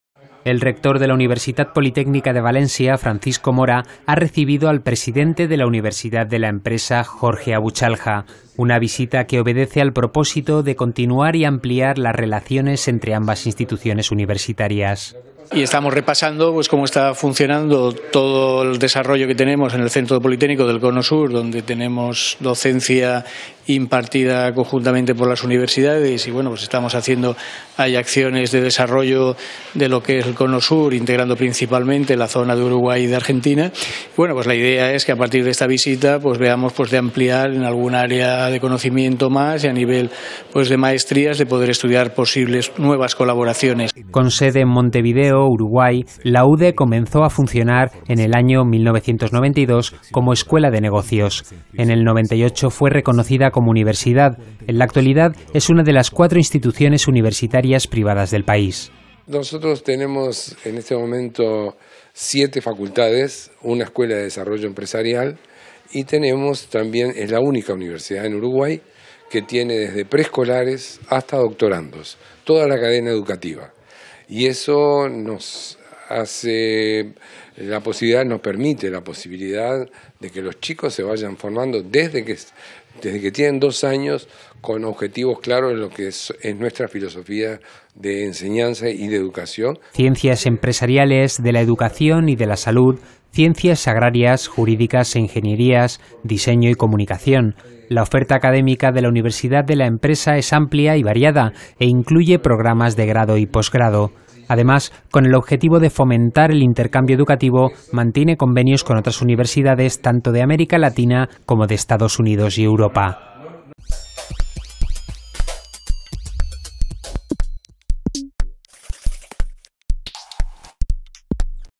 Noticia @UPVTV: Visita del rector Universidad de la Empresa [10-11-2015]
El rector de la Universitat Politècnica de València, Francisco Mora, ha recibido al presidente de la Universidad de la Empresa, Jorge Abuchalja. Una visita que obedece al propósito de continuar y ampliar las relaciones entre ambas instituciones universitarias.

Con sede en Montevideo, Uruguay, la UDE comenzó a funcionar en el año 1992 como Escuela de Negocios. En el 98, fue reconocida como Universidad. En la actualidad, es una de las cuatro instituciones universitarias privadas del país.

Ciencias Empresariales, de la Educación y de la Salud, Ciencias Agrarias, Jurídicas e Ingenierías, Diseño y Comunicación. La oferta académica de la Universidad de la Empresa es amplia y variada, e incluye programas de Grado y Posgrado.

Además, con el objetivo de fomentar el intercambio educativo, mantiene convenios con otras universidades, tanto de América Latina como de Estados Unidos y Europa.